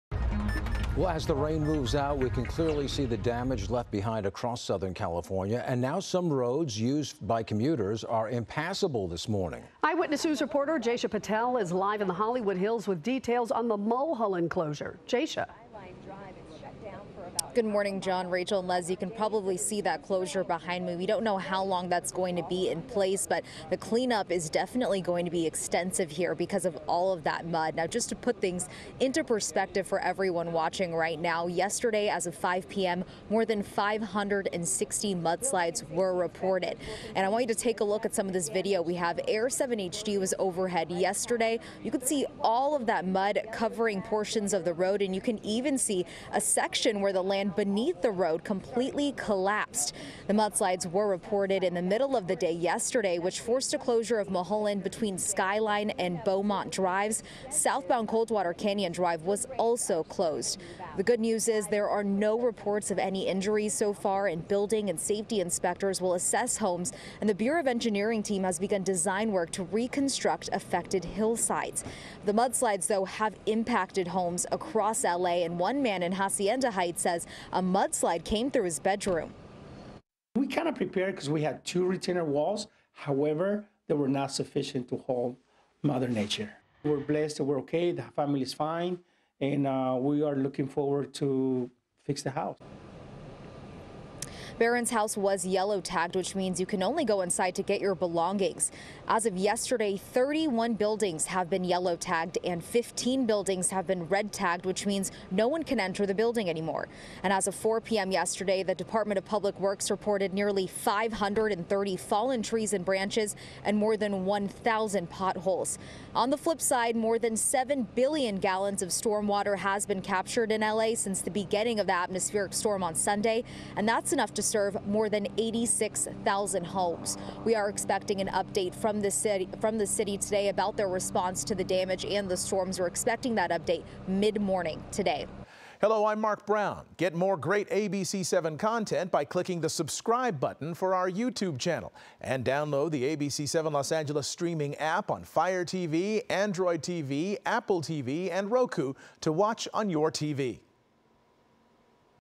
Stretch of Mulholland Drive in Studio City area remains closed after mudslides
The city reported mudslides and washouts near the 8600 to 8700 block of Mulholland between Bowmont and Skyline drives, which remained closed Thursday morning.